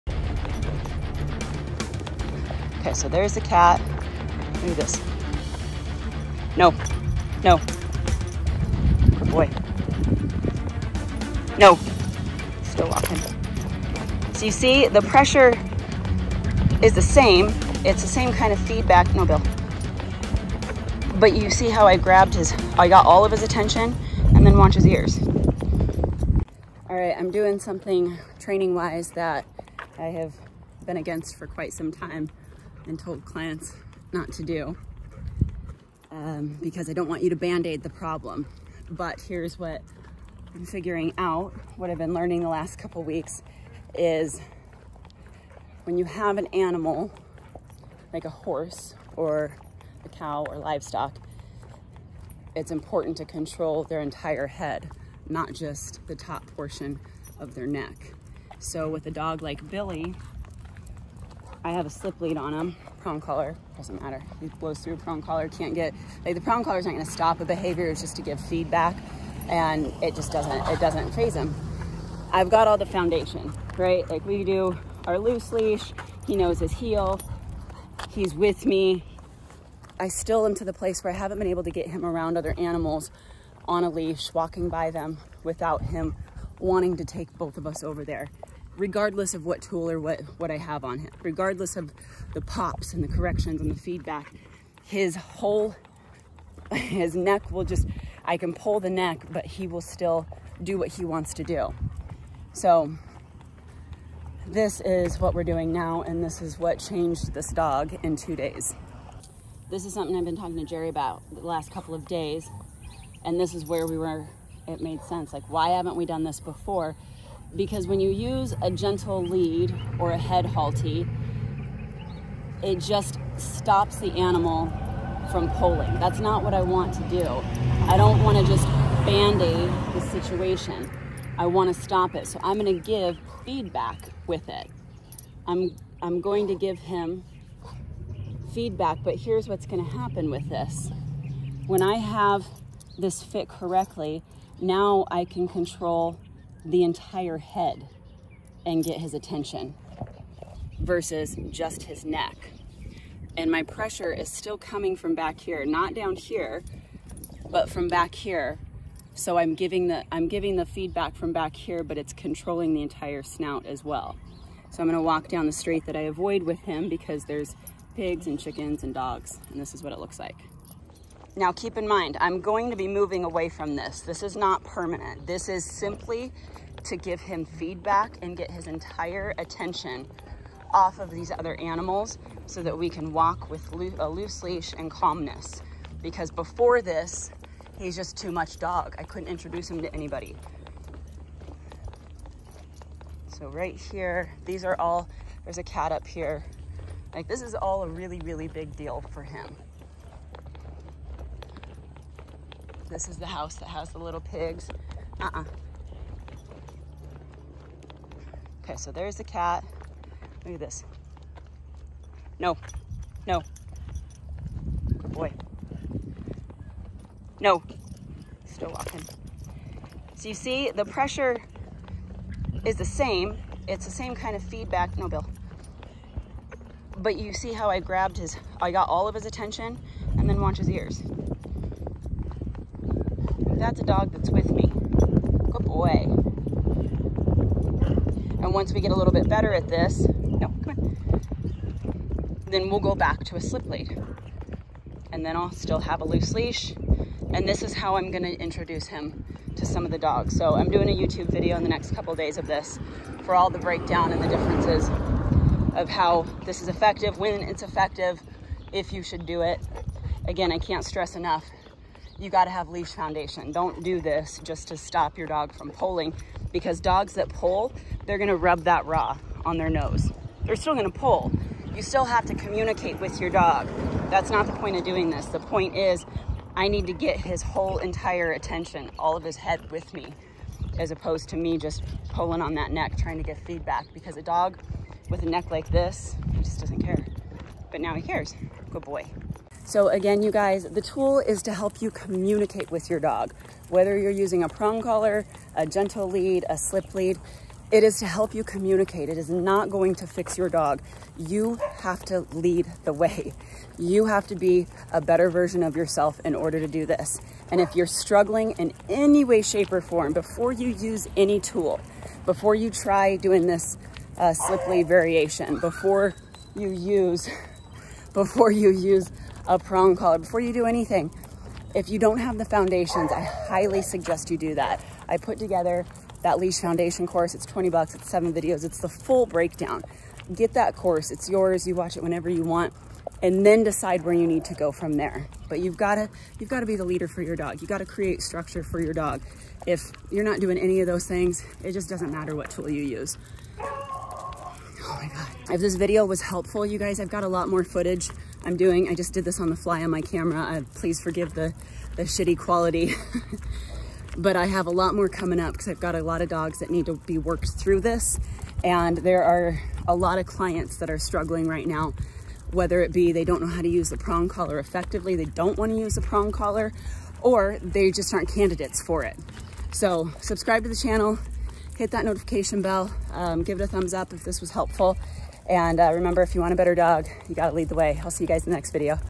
How to stop extreme pulling(NO PRONG!!)
This dog was far too dominant and pushy for a prong collar. He blew right thru it repeatedly, even with foundation, structure, training, feedback, handfeeding... I couldnt seem to have any real break throughs, until I realized I needed to get control of his WHOLE head, not just his neck.

If you need help with the foundation, go here:
LeashFoundation.com

HandoverRover.com